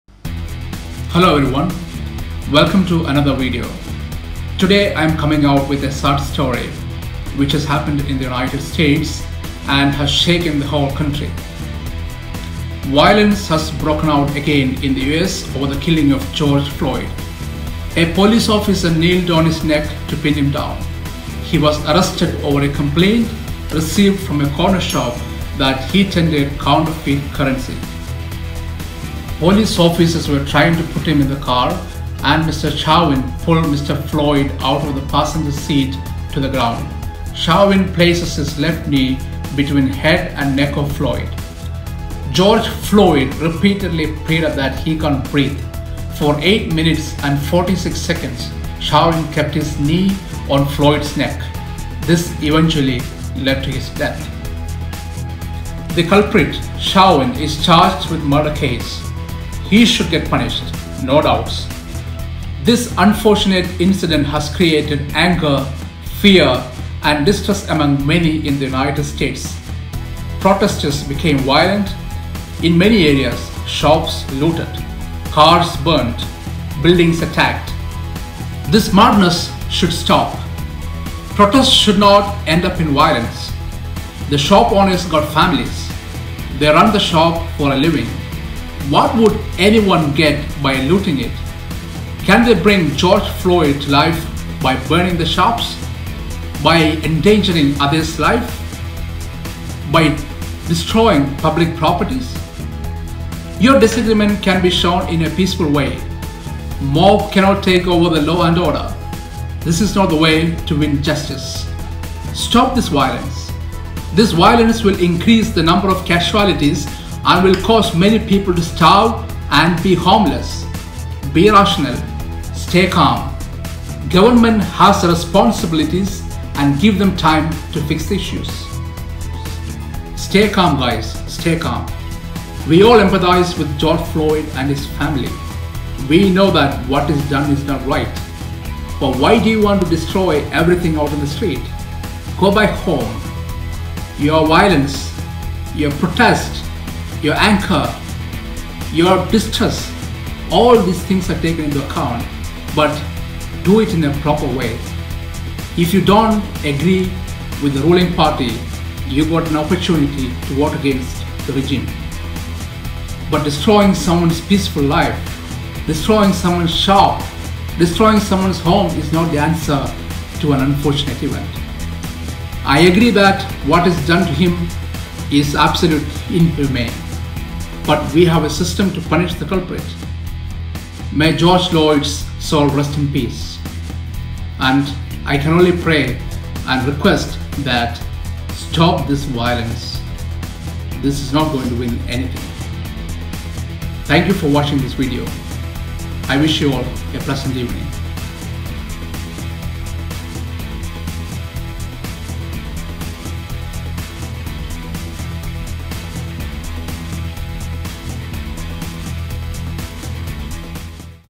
What is happening in the US now? What happened to George Floyd?
A police officer named Chauvin kneeled on the neck of George Floyd while arresting him. For 8 minutes and 46 seconds Chauvin kept his knee on Floyd's neck resulting in his death.
There are protests happening almost every state in the US. People are looting shops, destroying cars and buildings. This madness should stop. This unrest will not solve anything nor bing George Floyd back to life.
This is a call for peace. Stop violence. Stay Calm.
The loss of Floyd is beyond words. We empathise with his family. May his soul rest in peace.